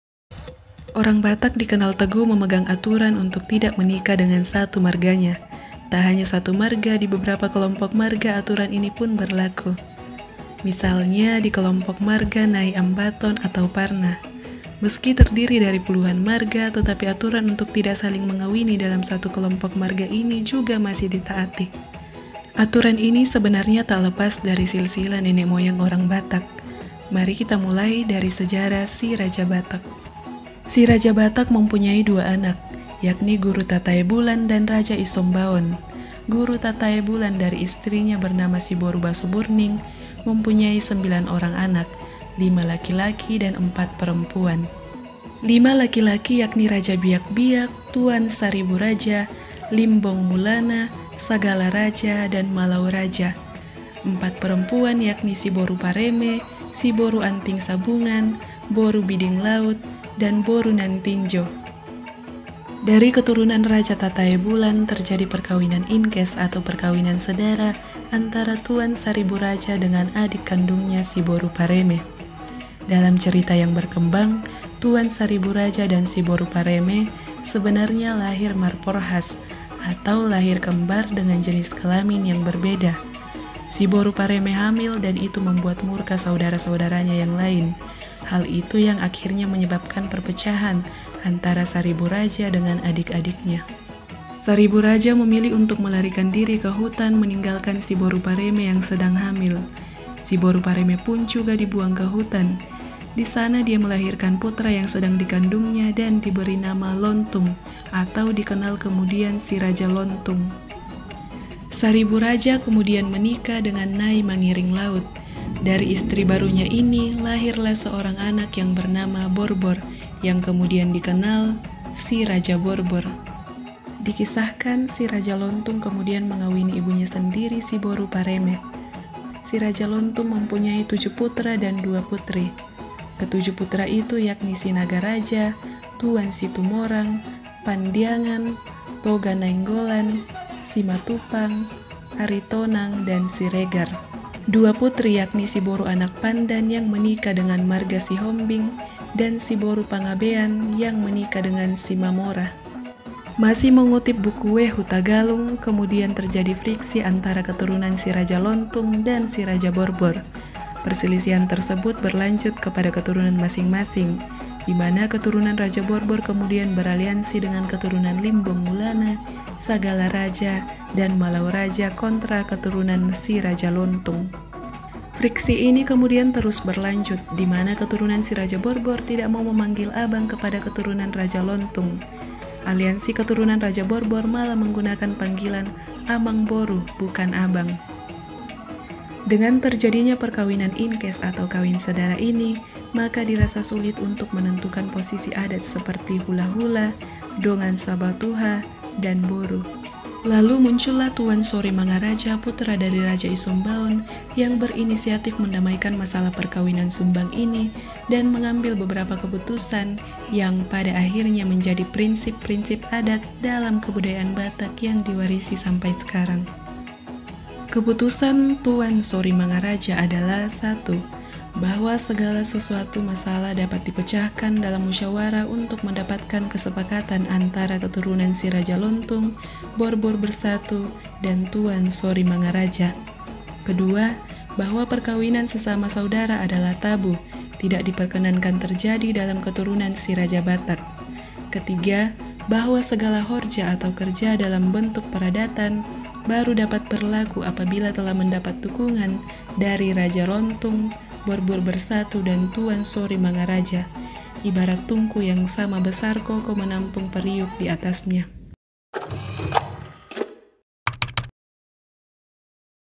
Kenapa Orang Batak Tak Boleh Kawin Semarga? Begini Sejarahnya | Opsi.id
Orang Batak dikenal teguh memegang aturan untuk tidak menikah dengan satu marganya. Tak hanya satu marga, di beberapa kelompok marga, aturan ini pun juga berlaku.

Misalnya, di kelompok marga Nai Ambaton atau Parna, meski terdiri dari puluhan marga, tetapi aturan untuk tidak saling mengawini dalam satu kelompok marga ini juga masih ditaati.